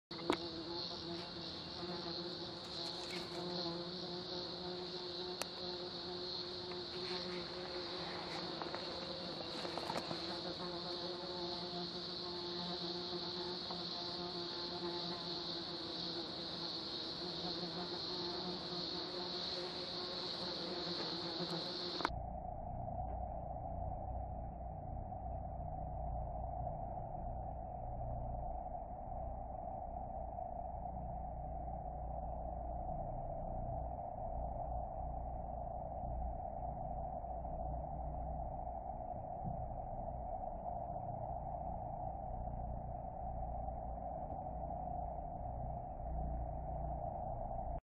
Flying Duck Orchids closing up
A video of how a Flying Duck orchid reacts when something lands on top of it